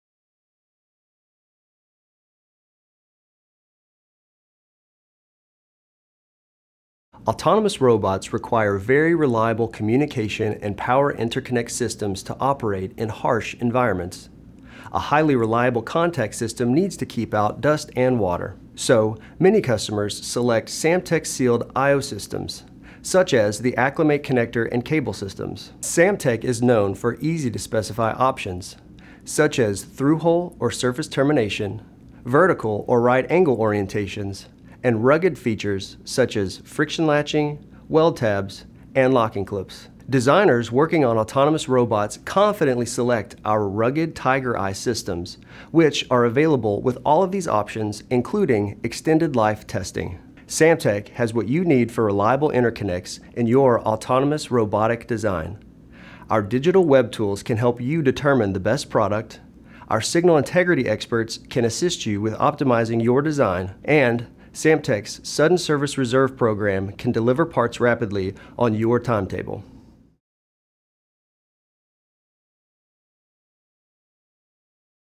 Samtec Solutions for Autonomous Robotics
Autonomous robots are helping to streamline supply chain operations with increased speed and improved accuracy. When coupled with AI, autonomous robotics have a higher level of decision making for data collection, which helps reduce costs and boost productivity. Samtec offers micro rugged and high-density interconnects to support reliable communication and power transmission in these autonomous systems, even in challenging environments.